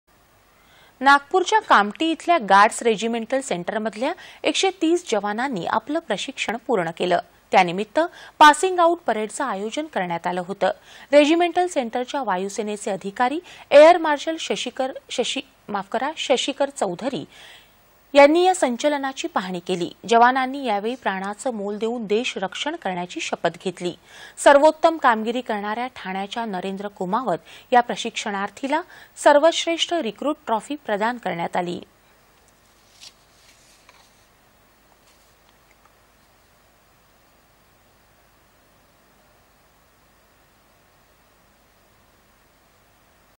नागपूर: कामटी गार्डस् रेजिमेंटल सेंटरमधल्या १३० जवानांच प्रशिक्षण पूर्ण
दि.१८.११.२०२१ :
         नागपूरच्या कामटी इथल्या गार्डस् रेजिमेंटल सेंटरमधल्या १३० जवानांनी आपलं प्रशिक्षण पूर्ण केलं. त्यानिमित्त पासिंग आऊट परेडचं आयोजन करण्यात आलं होतं. रेजिमेंटल सेंटरच्या वायुसेनेचे अधिकारी एअर मार्शल शशिकर चौधरी यांनी, या संचलनाची पाहणी केली. जवानांनी यावेळी प्राणाचं मोल देऊन देश रक्षण करण्याची शपथ घेतली. सर्वोत्तम कामगिरी करणाऱ्या ठाण्याच्या नरेंद्र कुमावत या प्रशिक्षणार्थीला सर्वश्रेष्ठ रिक्रुट ट्रॉफी प्रदान करण्यात आली.